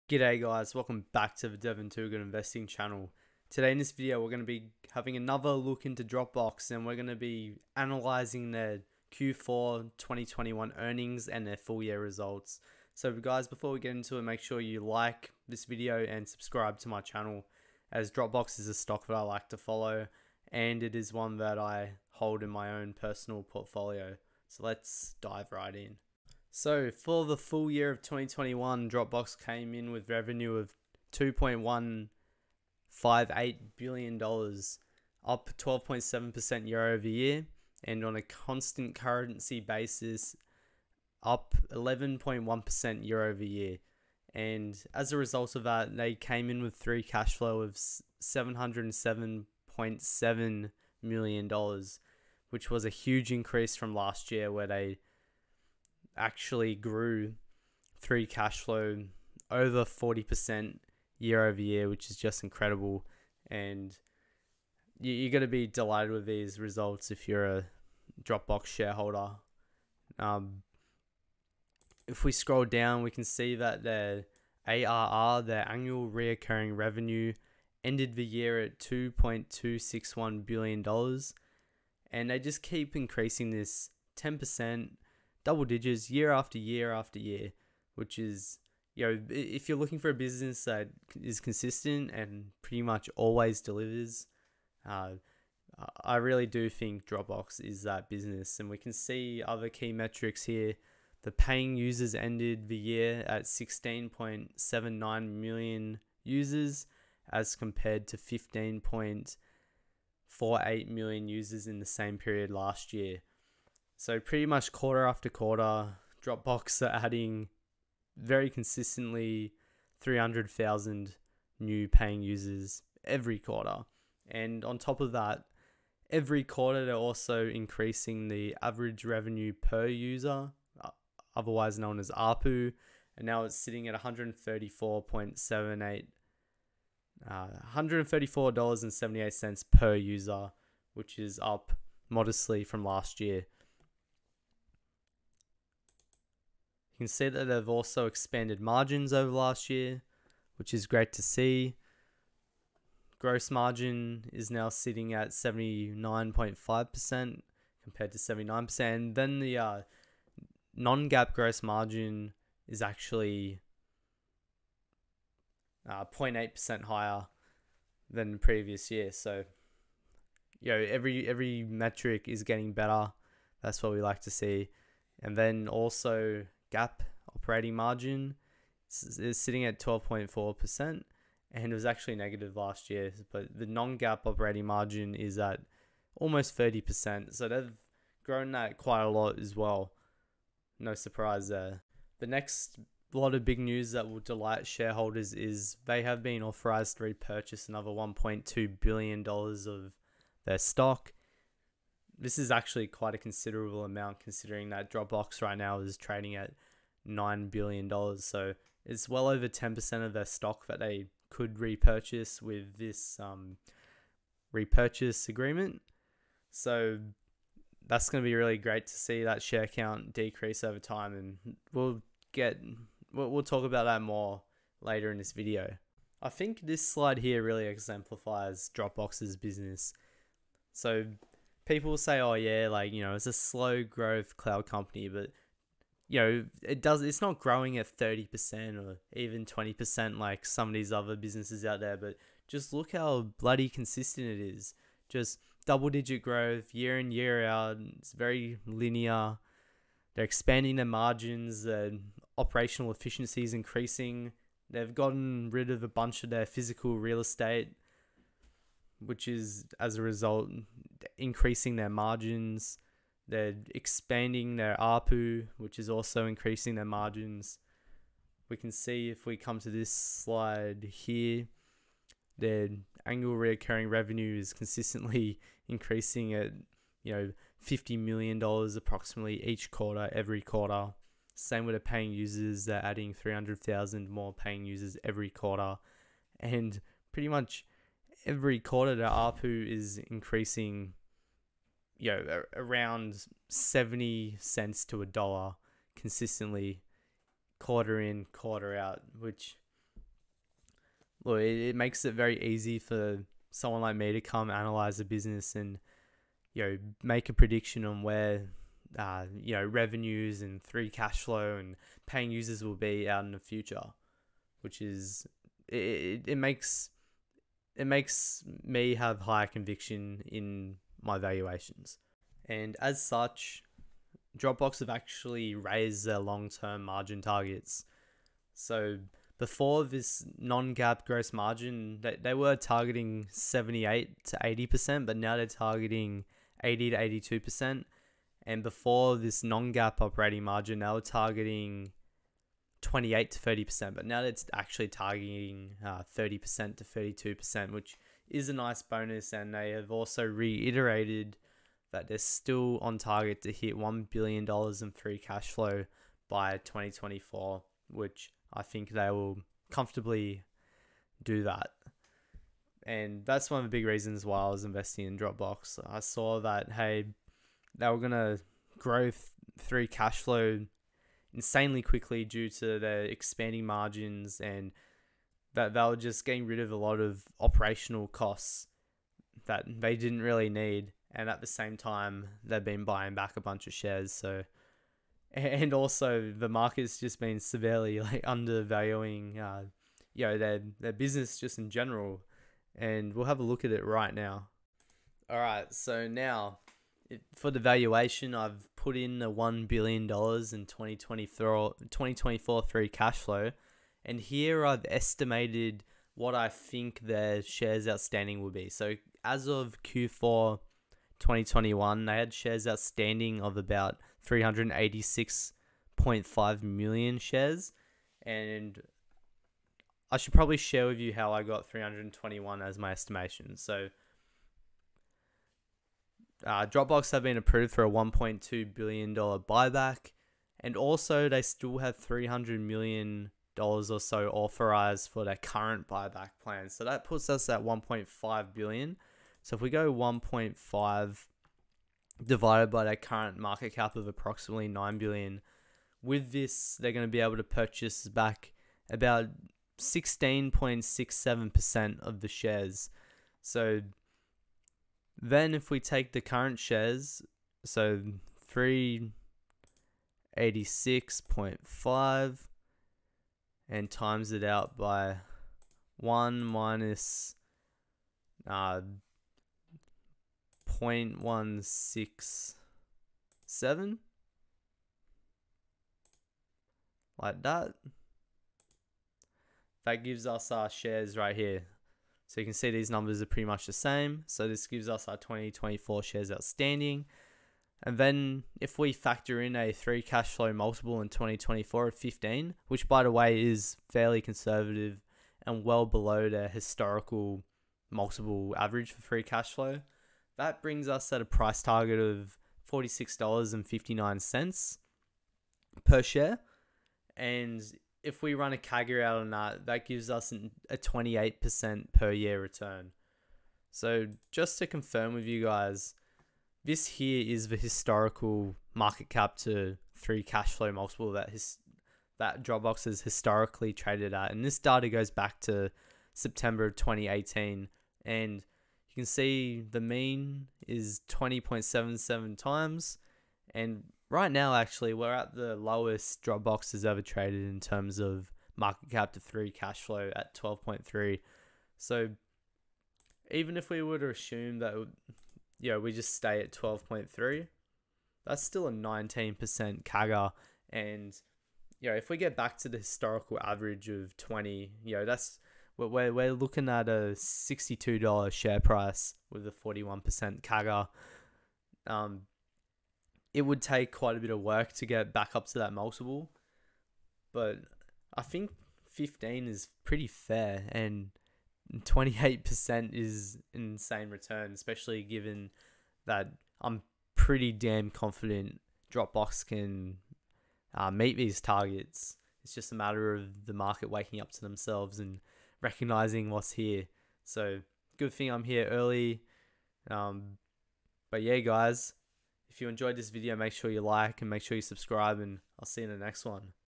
Dropbox Stock Q4 2021 Earnings Update and Intrinsic Valuation
In this video, we look at DBX stock Q4, 2021 earnings. I think the shares will be worth about $46 per share in 2024.